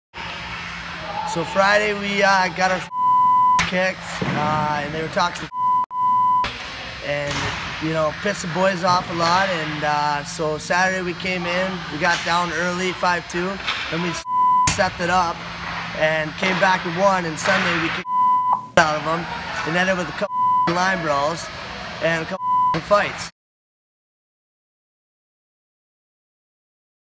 Hockey Talk with ElimiShield SPORT
Jay Croop, forward for the Carolina Thunderbirds, gives us the scoop on their recent win.